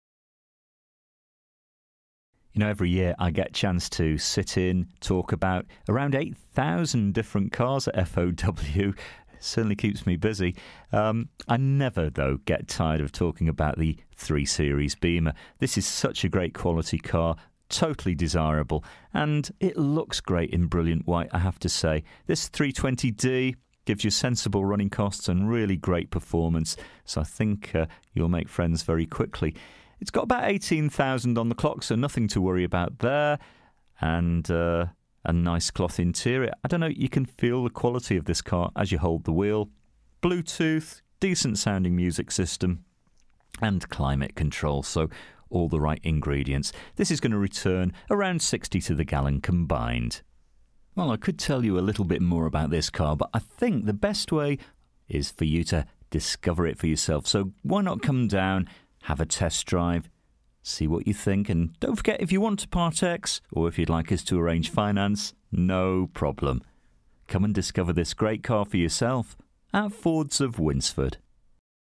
BMW 3 SERIES DIESEL SALOON (2012) 320D SE 4DR - MF12CTY
DETAILS:
First registered: 02/03/2012
Current mileage: 18010
Registration: MF12CTY
Colour: WHITE

FEATURES:
all round electric windows, remote central locking, cloth interior, all round airbags, climate control, pas, abs, alloy wheels, solid paint, cd, traction control, parking control, hand book present, cruise control, bluetoot

This used car is for sale at Fords of Winsford Car Supermarket in Cheshire, UK